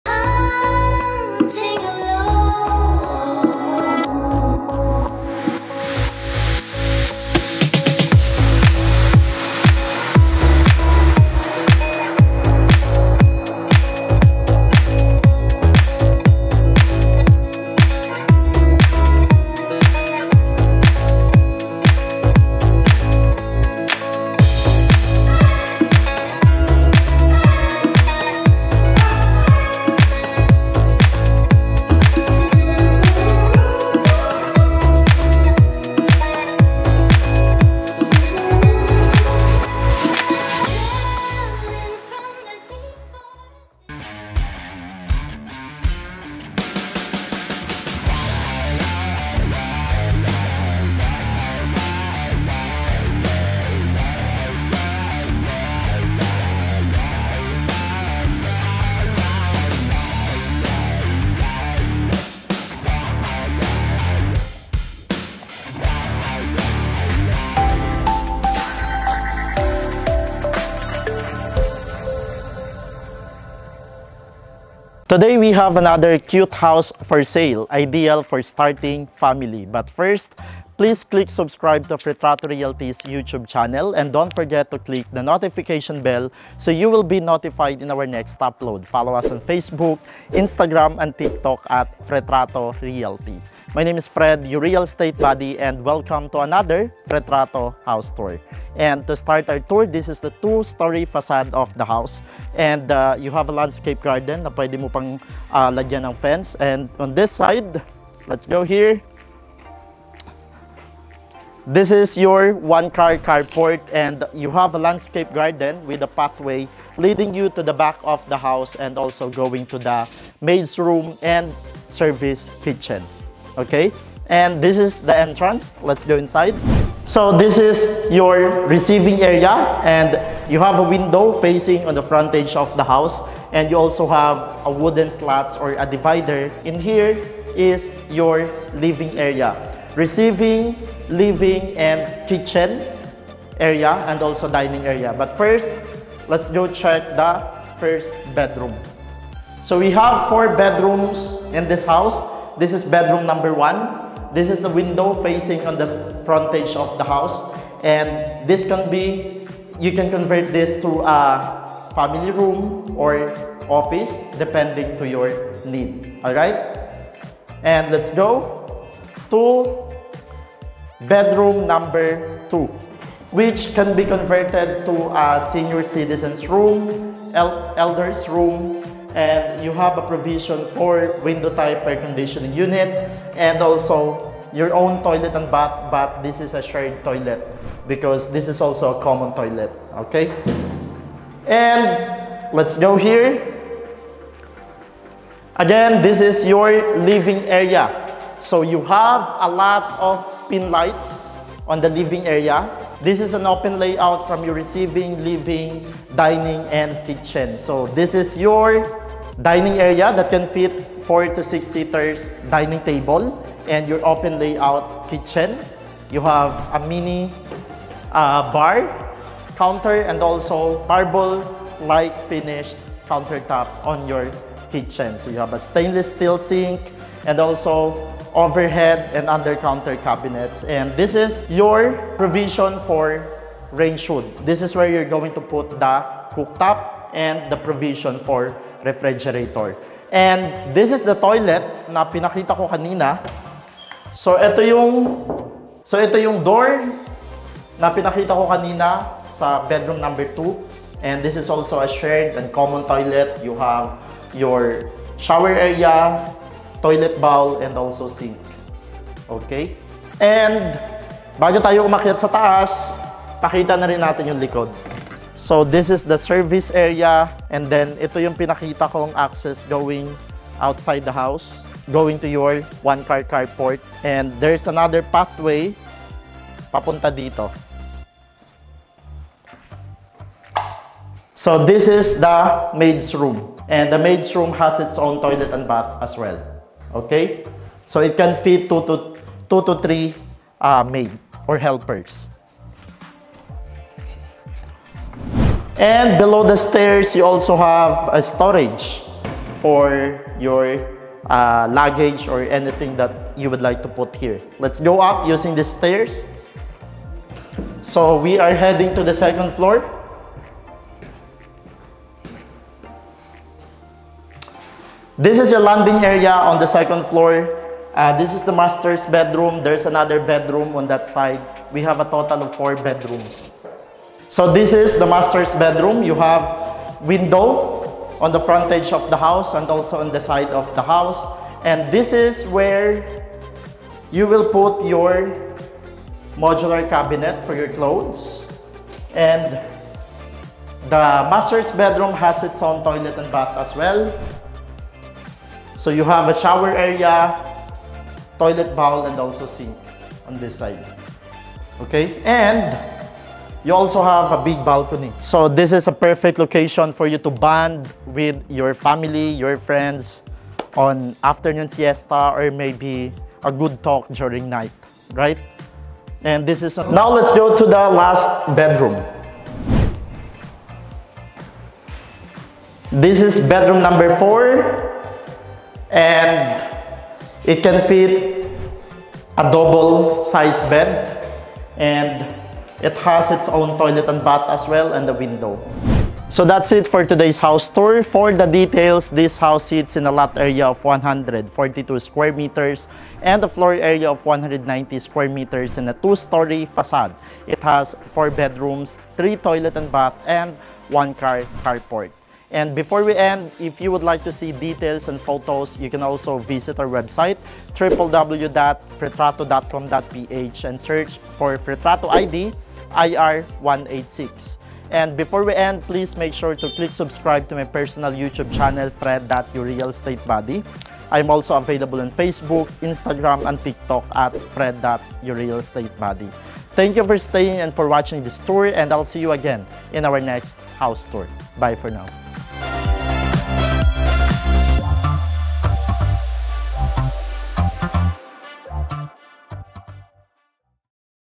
Inside a 13.8 Million Cute House for Starting Family • Fil-Invest East Cainta Rizal
FOR SALE
Filinvest East Homes
Cainta, Rizal

Lot Area: 142 square meters
Floor Area: 190 square meters
Floor Level: 2 Storey
Property ID: IR186

Bedrooms: 4
Toilet and Bath: 3
Inclusions: Unfurnished
Parking: 1 Car Garage

Key Features:
 -1 Spacious Balcony
 -Solid Concrete
-Gated and Guardhouse

List of Amenities inside the village:
 -Gated and Guardhouse
 -Swimming Pool and kiddie pool
 -Kid’s Park
 -Clubhouse
 -Red Keep Coffee Shop
 -Covered Basketball Court
 -Event’s Place inside Red Keep Facility

Selling Price: PHP 13,800,000

Fredlon A. Mellendrez - Fretrato Realty